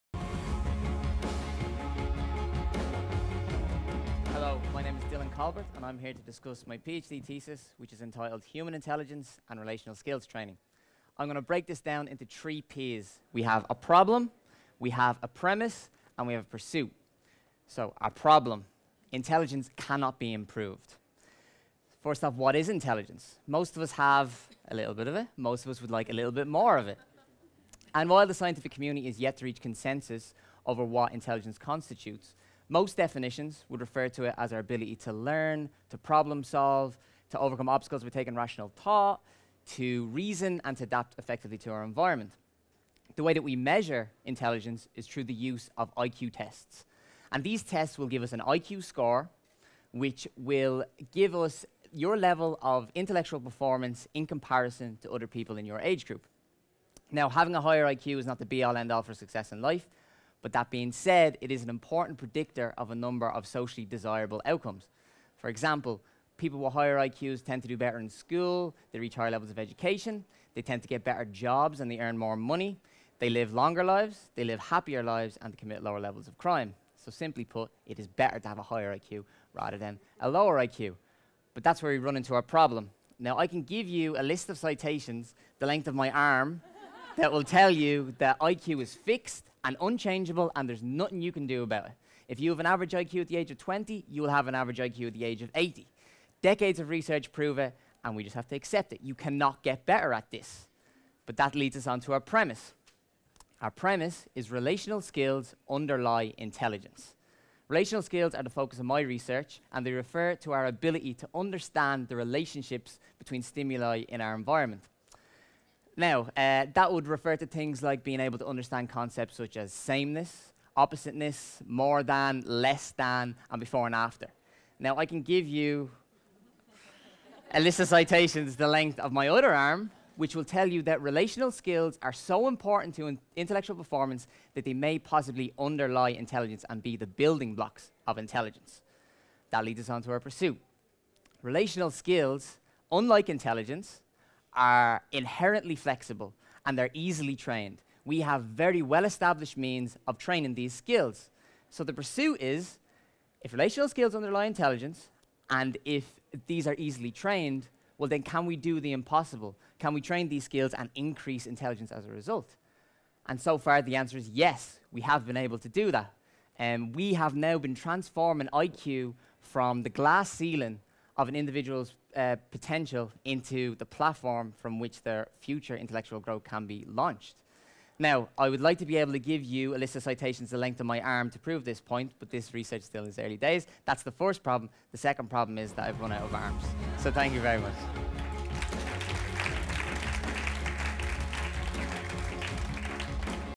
Researchfest 2017 finalist: Dylan Colbert | Inspirefest 2017
Dylan Colbert wants to help us get smarter. The Maynooth University researcher explains how he intends to achieve that during his presentation at Researchfest 2017.

for the latest event and ticket information.